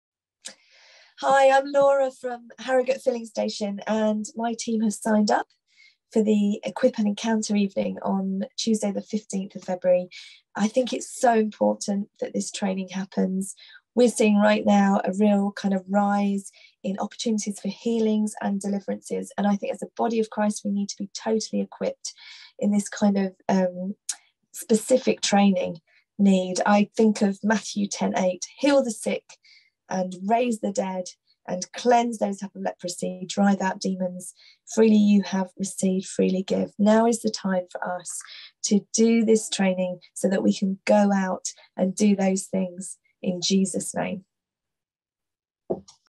Sign up for "Deliverance Equip & Empower"
Would you like to minister, as Host Teams, with confidence in the kingdom authority that Jesus has given you? Then join our practical evening of training on deliverance and prayer: "Deliverance Equip & Empower".

Laura Brett (Harrogate Filling Station) shares why her team have signed up!

7:30-9pm, Tuesday 15 February

- How to shift atmospheres in our venues
- What happens if there is a demonic manifestation in a prayer ministry time?
- Engaging in spiritual warfare wisely
- Biblical framework of deliverance ministry
- The importance of focus on Jesus
- Shifting atmospheres
- Discerning spirits
- Removing access points
- Q&A!